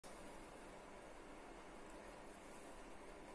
Anemone by Yvonne Wells
Anemone shaped Vase crafted from aged  roofing iron by Yvonne Wells Buninyong Australia